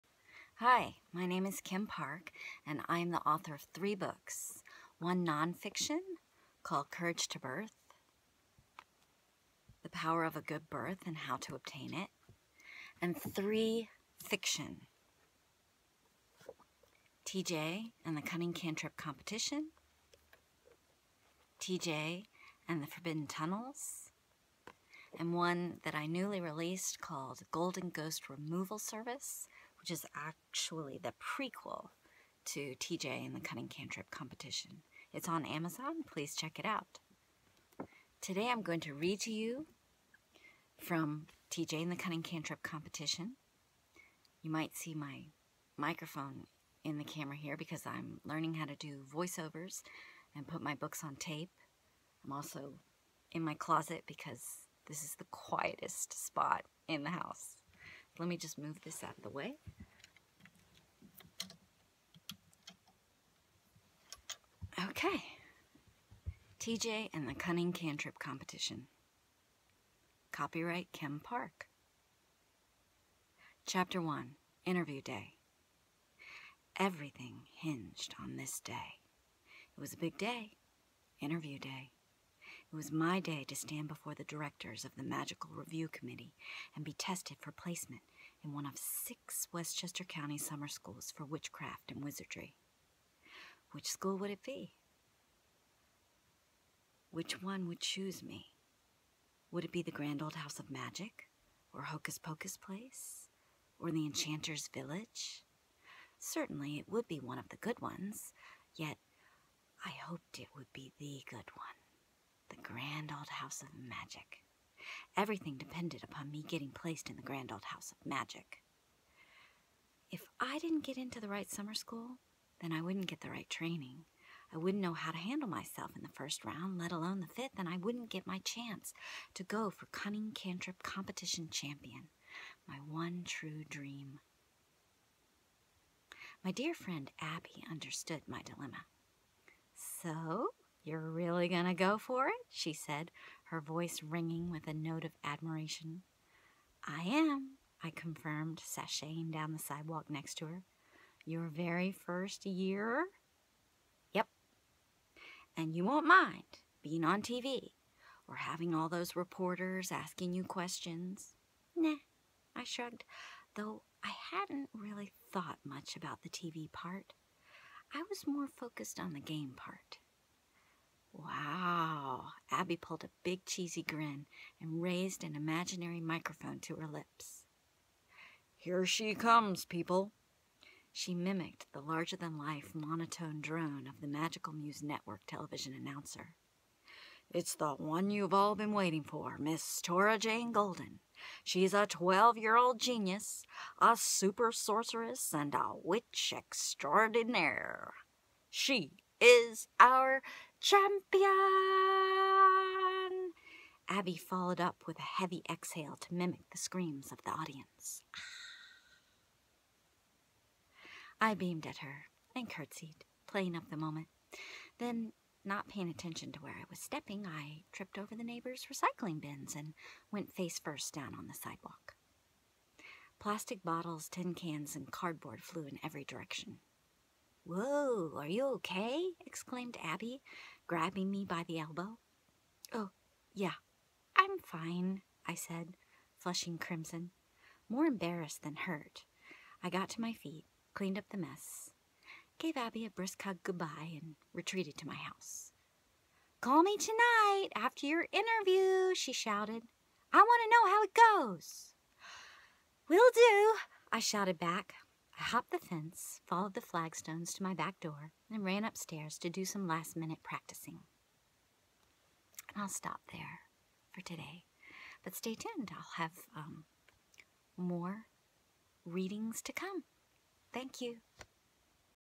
Reading 1.1 TJ & THE CUNNING CANTRIP COMPETITION
Follow along and listen to the variety of voices Kim Park creates as she reads from her book, TJ & THE CUNNING CANTRIP COMPETITION